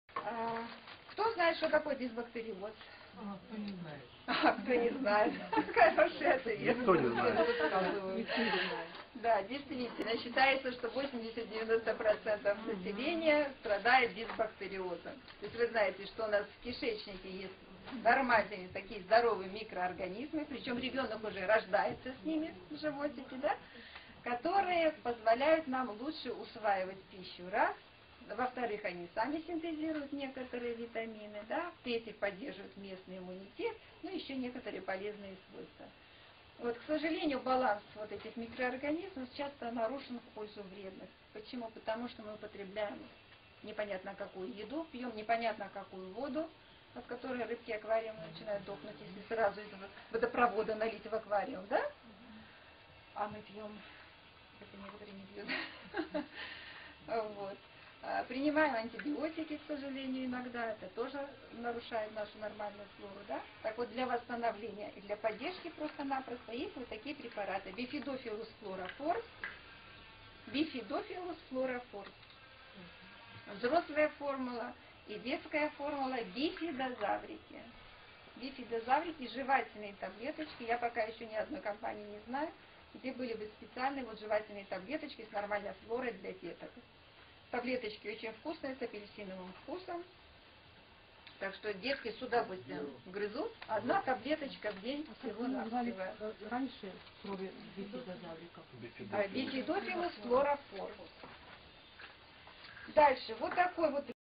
Бифидофлора NSP
Фрагмент лекции врача нутрициолога Смирновой Нины Павловны
статьи кандидатов медицинских наук и врачей-нутрициологов о продукции NSP можно посмотреть на сайте